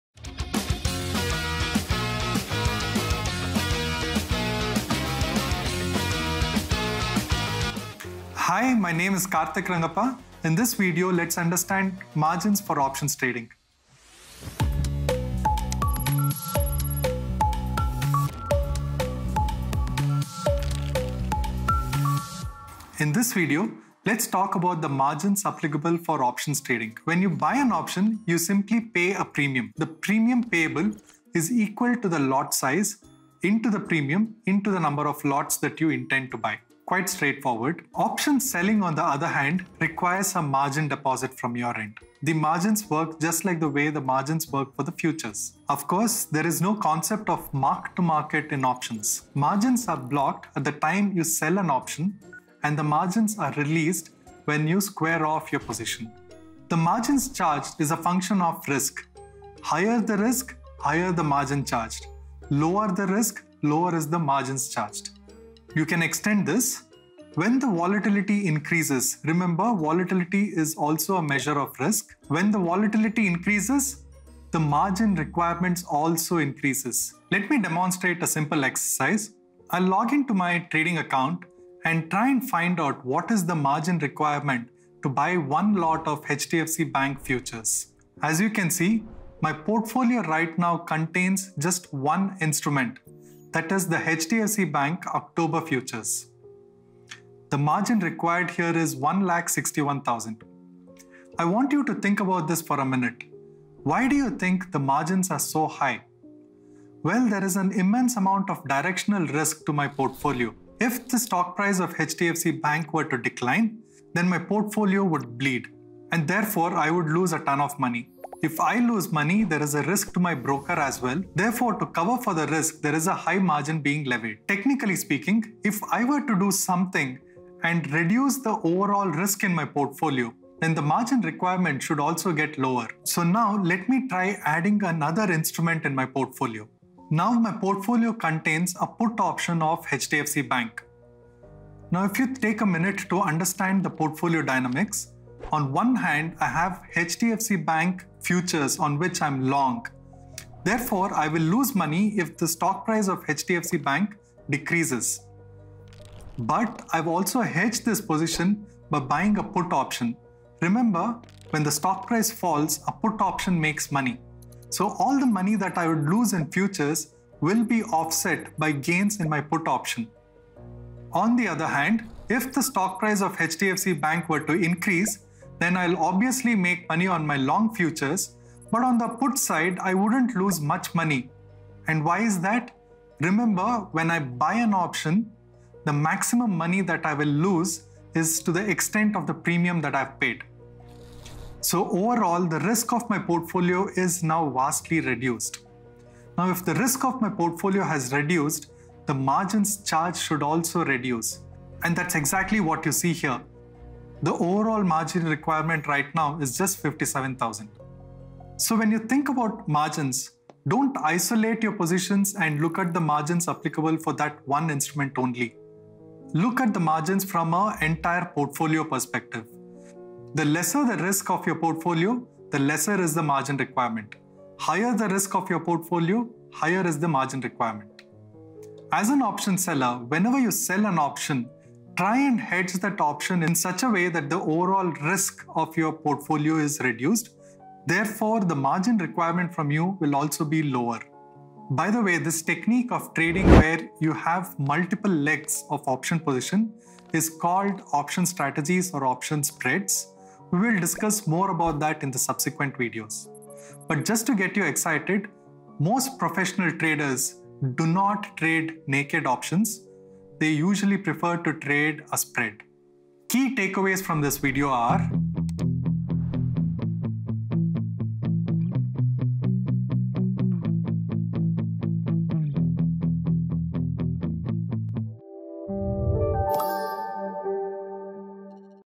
11. Margins
When trading options, you'll hear a term called "margin." It's nothing but the money required to buy or sell an option. Margin requirements are different for futures, call options and put options. They are also dynamic and change with volatility. Knowing how margins are calculated is important when trading options because it helps you deploy and manage your capital appropriately.

In this video, we look how margins are calculated for simple directional trades and when you hedge positions.